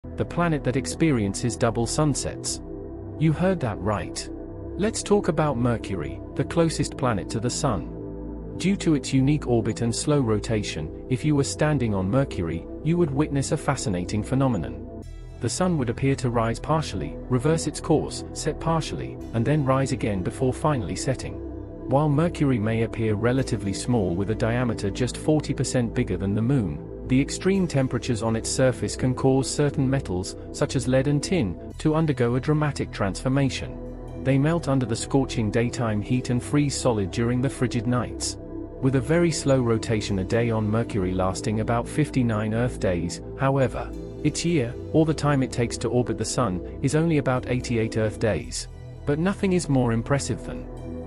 SPOILER: It's From Our Solar System...
The planet that experiences double sunsets…
You heard that right… Let’s talk about Mercury, the closest planet to the sun.
Due to its unique orbit and slow rotation, if you were standing on Mercury, you would witness a fascinating phenomenon.
The Sun would appear to rise partially, reverse its course, set partially, and then rise again before finally setting.
While Mercury may appear relatively small with a diameter just 40% bigger than the Moon, the extreme temperatures on it's surface can cause certain metals, such as led and tin, to undergo a dramatic transformation. They melt under the scorching daytime heat and freeze solid during the frigid nights.
With a very slow rotation a day on Mercury lasting about 59 Earth days.  However. Its year, or the time it takes to orbit the Sun, is only about 88 Earth days.